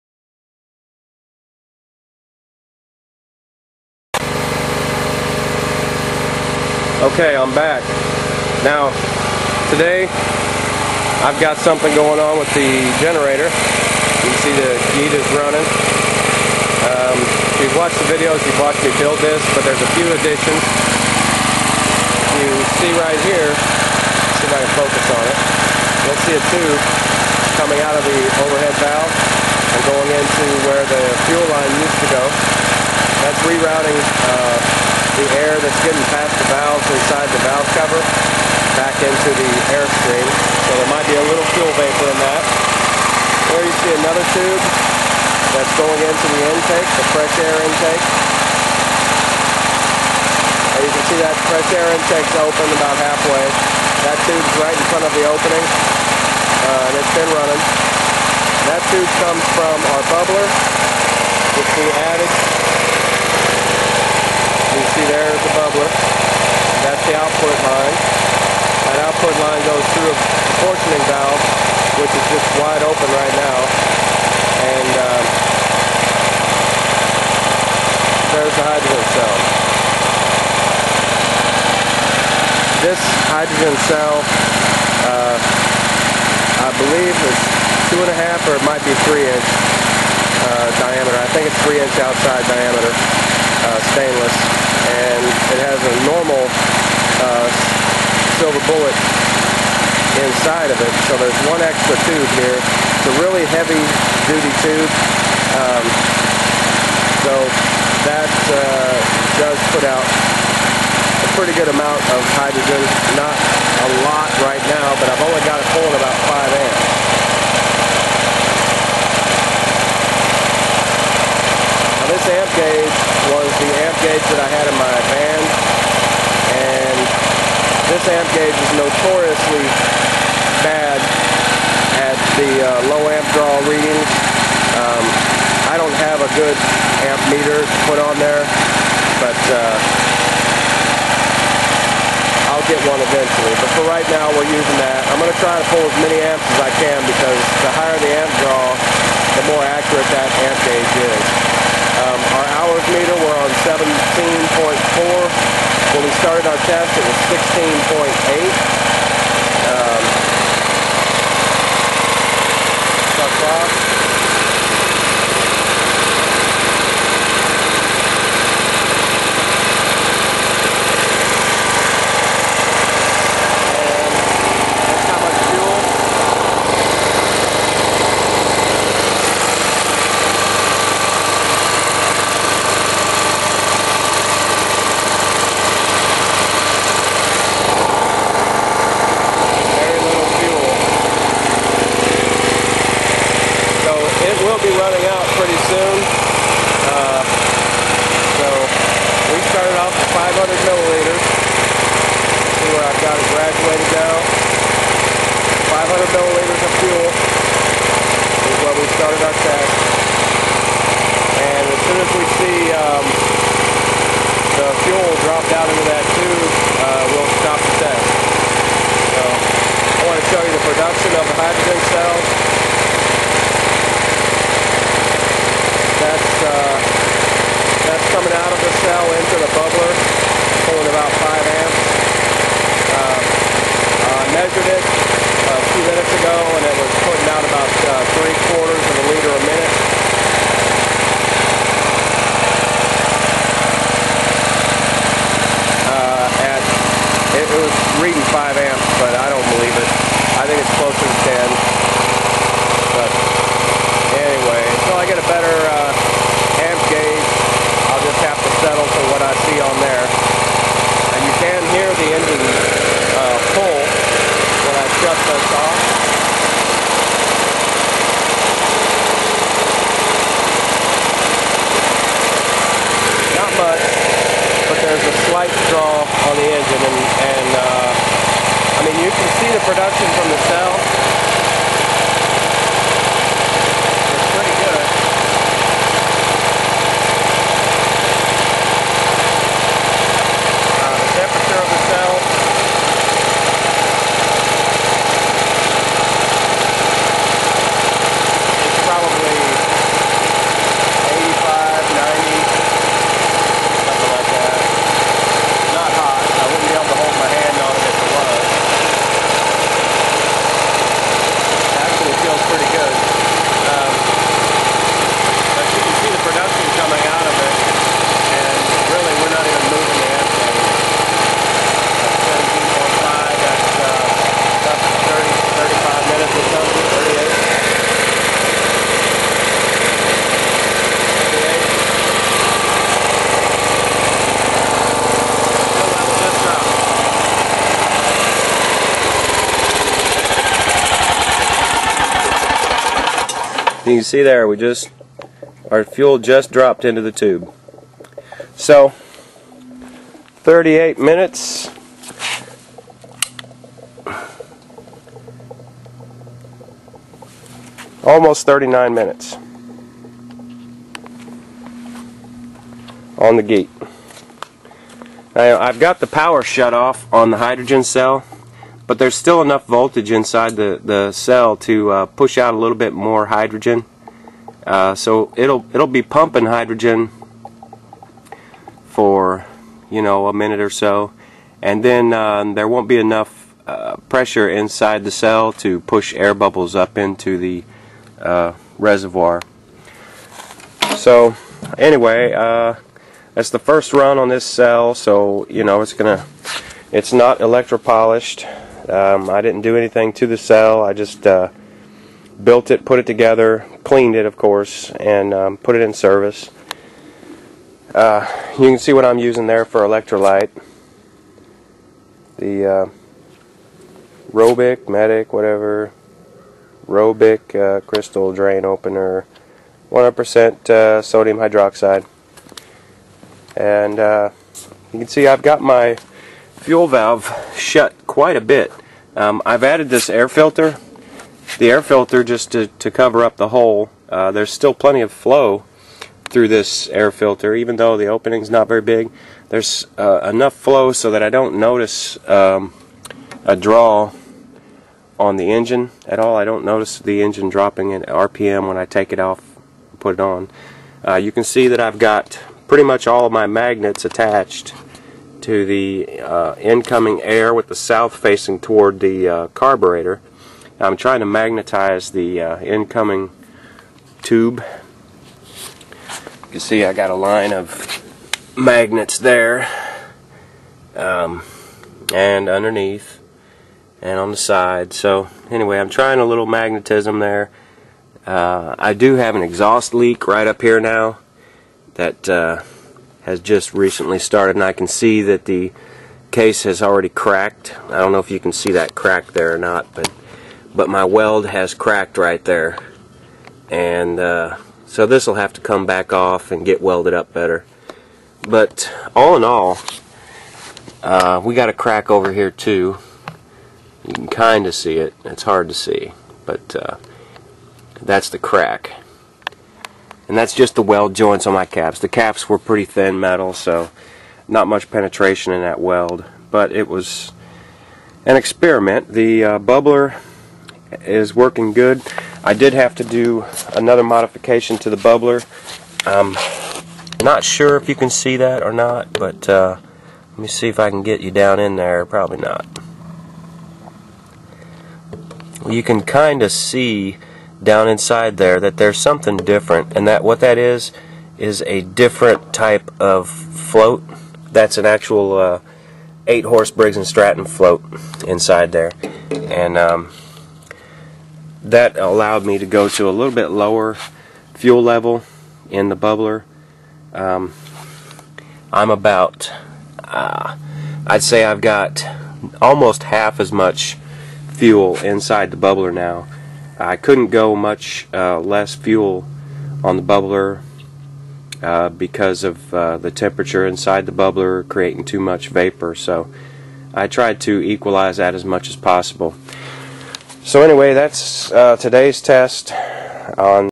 GEET Testing 1
This video shows a few new upgrades to the generator. It has Hydrogen added with the circuit capable of pulling 30 amps. The bubbler has a new float valve installed allowing a lower fuel level in the bubbler. This test was done with 500 ml of 87 octane gas. Run time was over 38 minutes. This time is nearly double what the generator ran on the same amount of fuel carbeureted. Next tests will be done with load and other variables such as more hydrogen and water vapor.